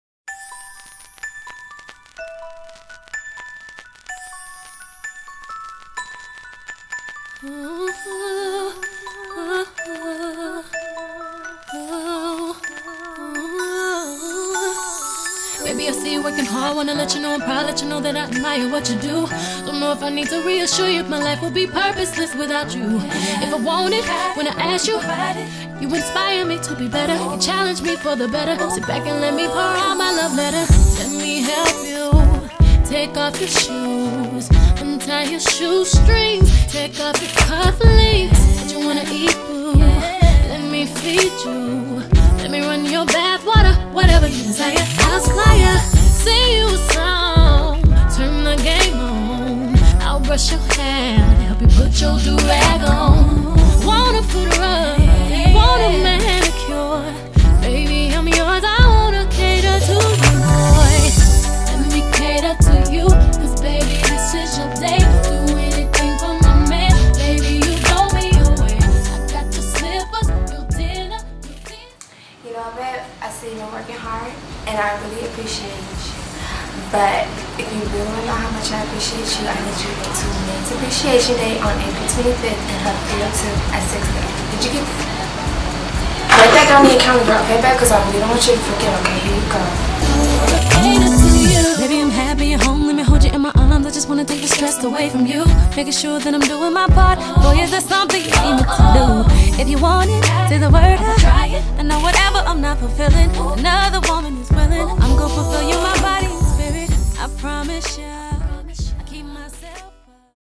S A S I Men's Appreciation Promo 2014
Sisters Against Social Injustice Promo for their "Men's Appreciation Night" happening April 25, 2014 7PM in HUB 355 *Watch in 1080p*